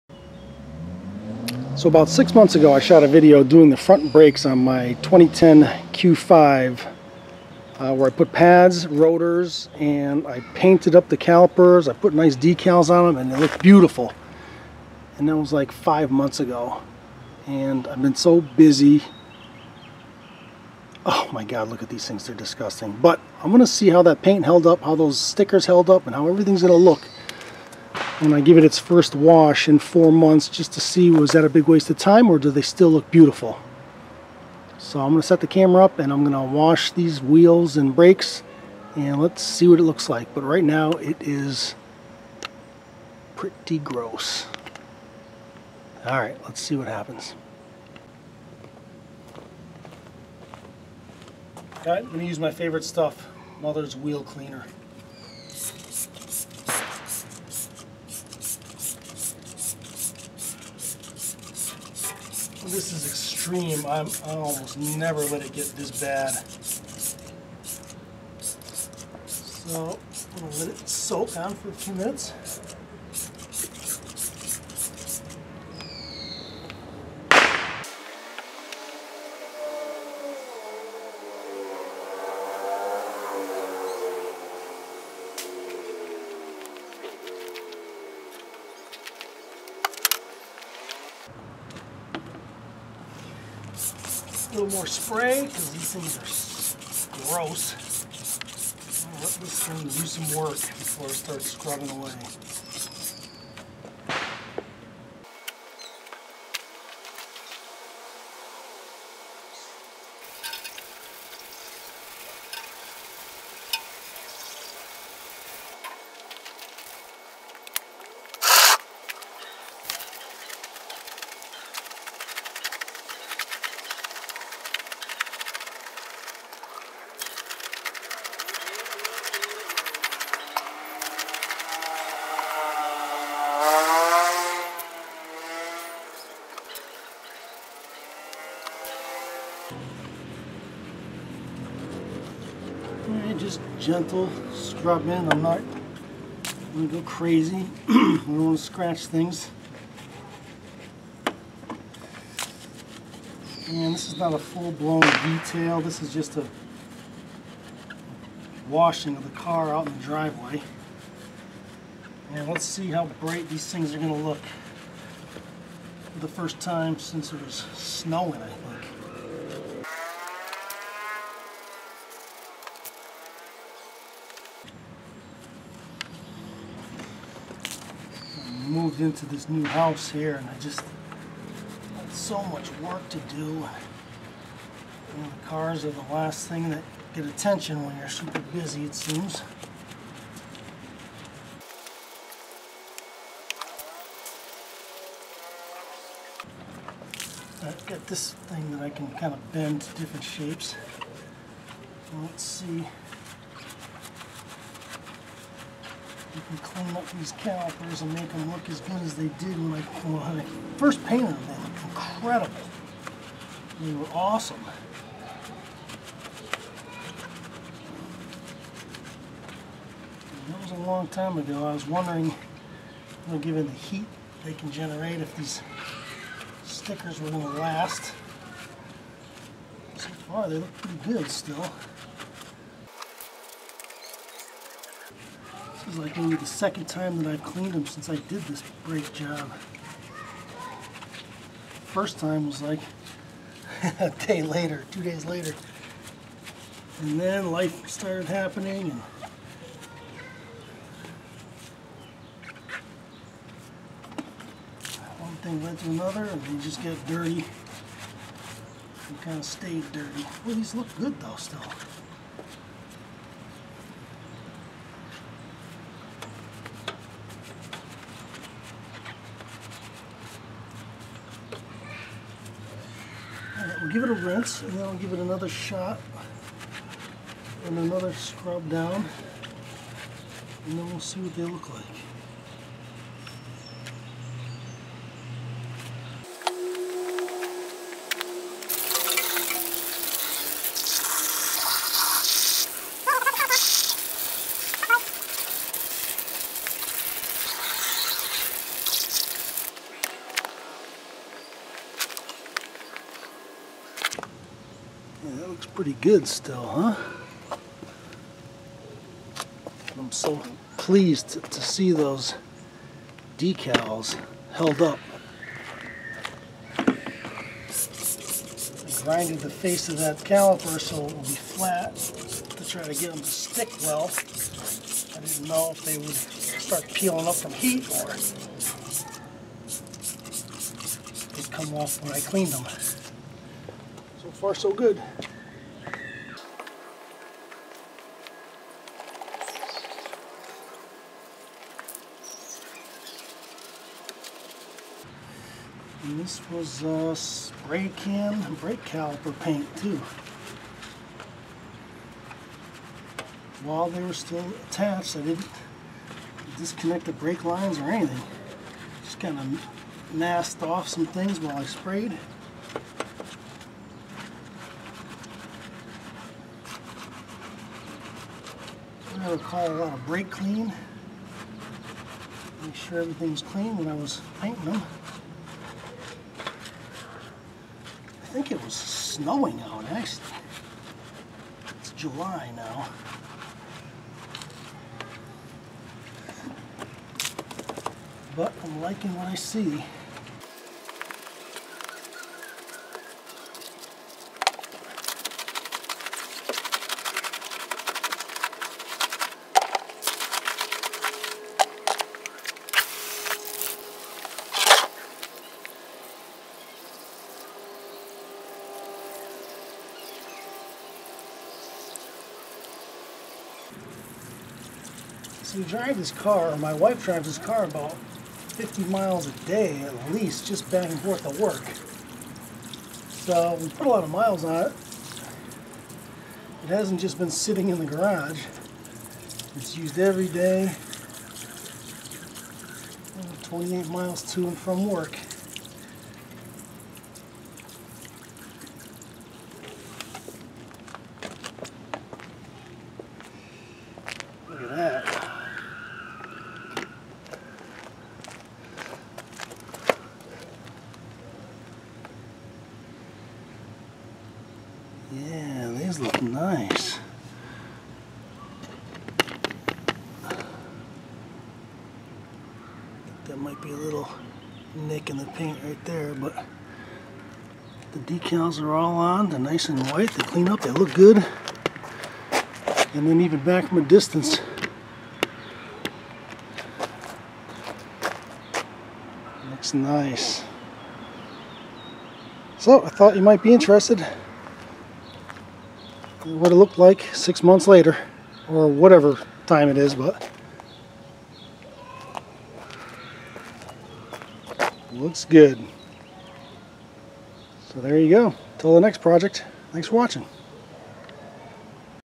Audi Q5 painted brakes 6 months later
How the painted brake calipers are holding up